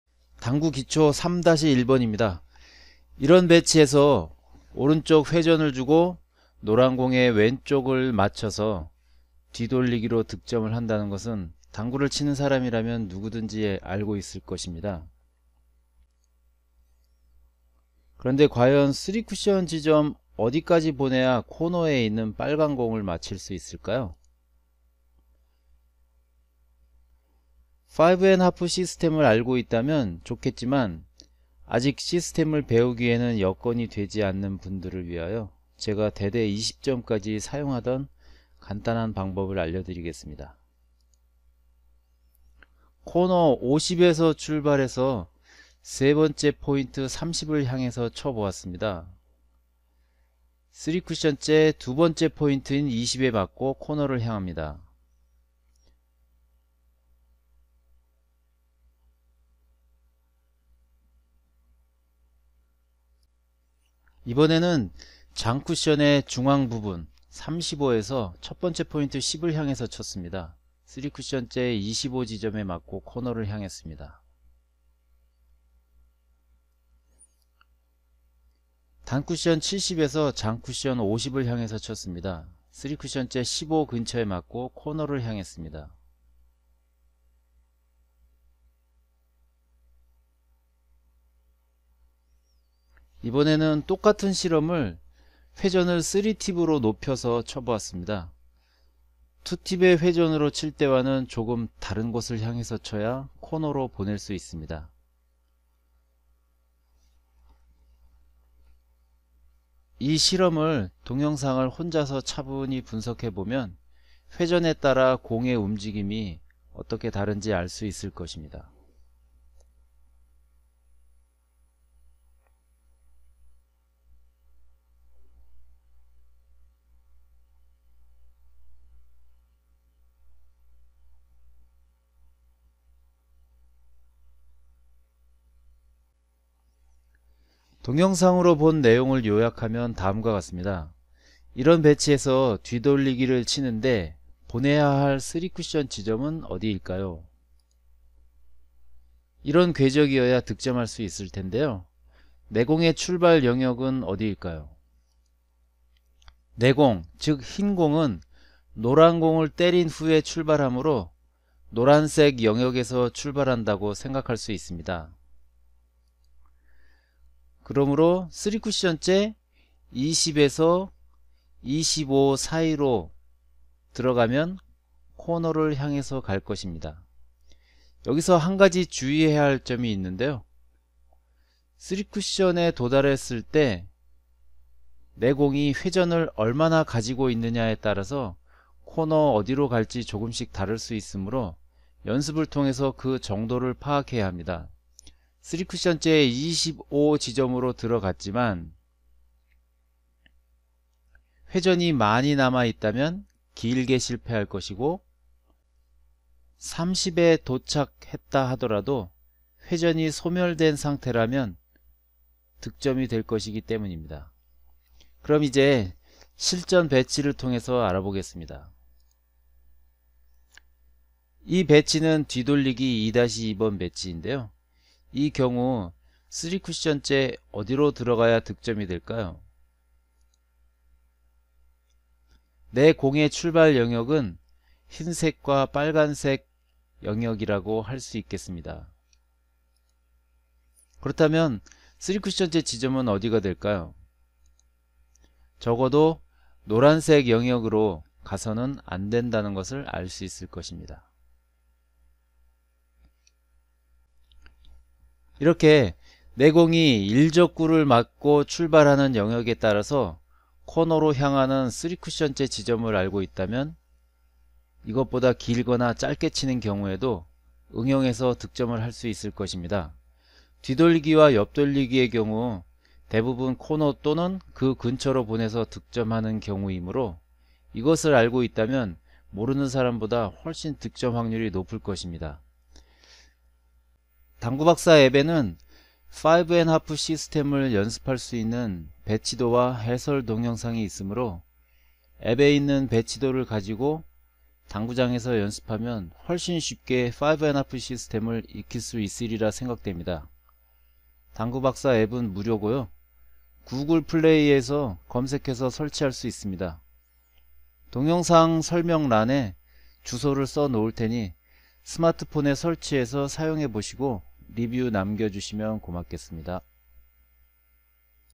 당구기초 3-1 코너로 보내는 간단한 시스템 (당구박사 쓰리쿠션 당구레슨)
* 뒤돌리기와 옆돌리기 등에서 내공을 코너로 보내기 위한 간단한 방법입니다.
이 내용은 당구박사의 스마트폰 당구교재 → 맞춤레슨 → 당구기초에 수록되어 있습니다.
■ '당구박사 당구레슨 마스터' (Master version download)
* for Android :